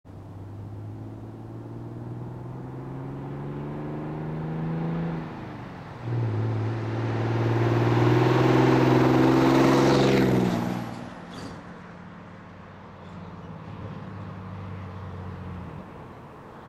Cummins Fly By With Trailer - 6.7L 6 Speed Manual Ram Regular Cab Dually
Attempting full throttle fly by (notice the clutch slipping) towing dump trailer with Bobcat.
2010 Dodge Ram 3500 6.7L Cummins, G56 6 Speed Manual, Regular Cab, Dually, 4x4, 3.42 rear gear
Mods: Smarty Jr (+30hp), Stock downpipe (has a cat) to 5 inch side exit exhaust with 6 inch tip, Airaid Intake